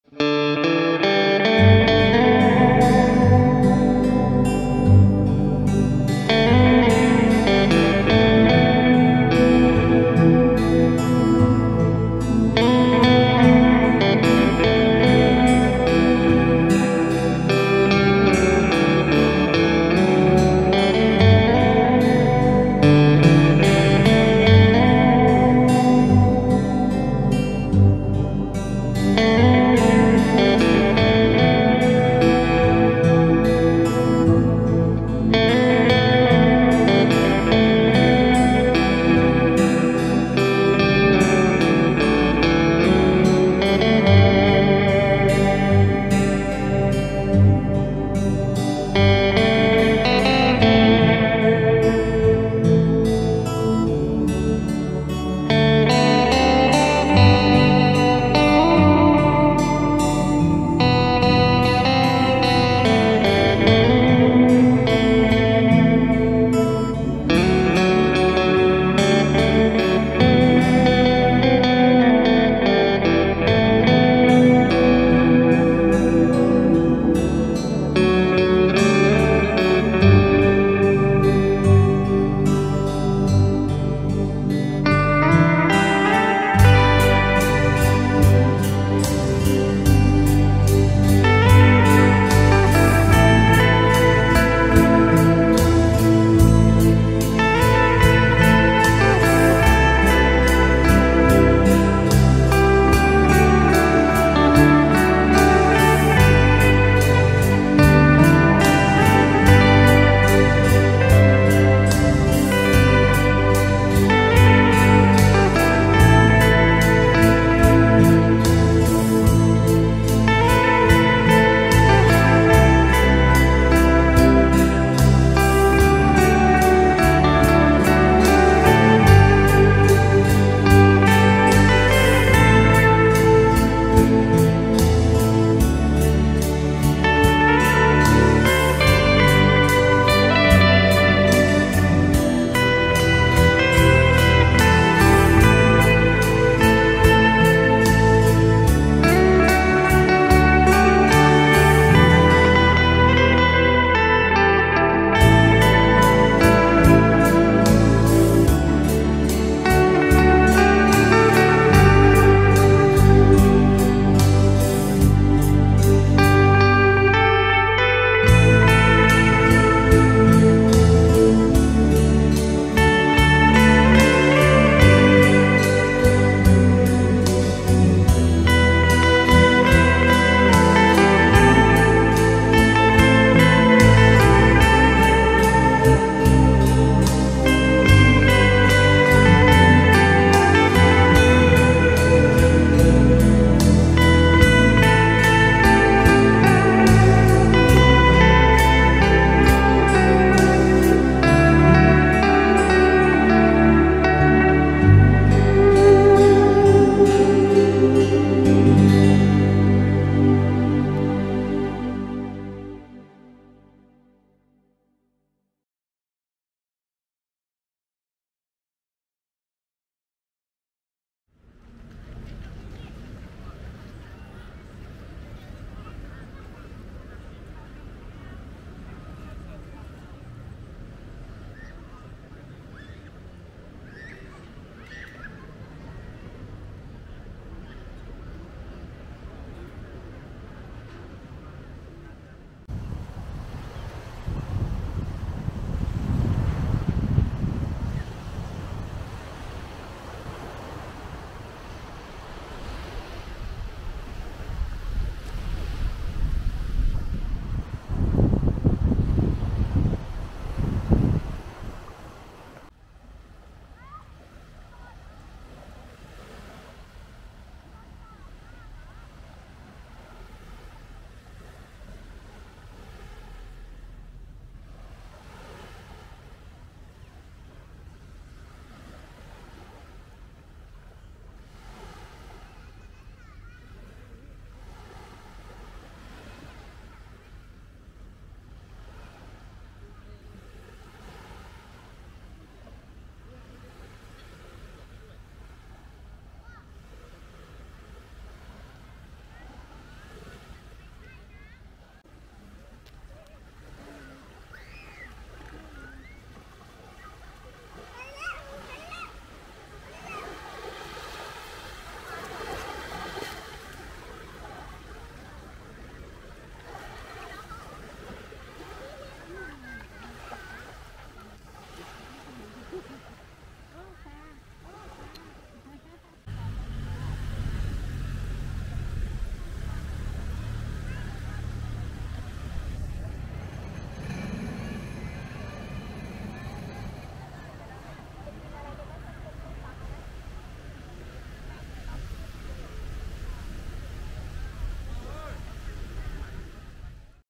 Prachuap Khiri Khan Bord de mer et plages 10 2023
Prachuap Khiri Khan province
*Pranmanee Beach Resort
161 Baanmai Moo.1 T. Samroiyod A.Samroiyod, 77120 Sam Roi Yot, Thaïlande
Offrant de belles vues sur la mer,
Situé sur une zone privée de la plage Sam Roi Yot Beach de Pranburi.
Vous pourrez louer un vélo, ou profiter des activités sur le plage, telles que le jet ski et le bateau banane.
*Khao Kalok Beach
Jolie petite plage.
On peut s'y restaurer,
nombreuses petites échoppes.
*Ao Manao Beach
Sur un terrain militaire
Nombreux restaurants
location d'une chaise longue pour une somme dérisoire (environ 50 ct d'euro)
Très agréable journée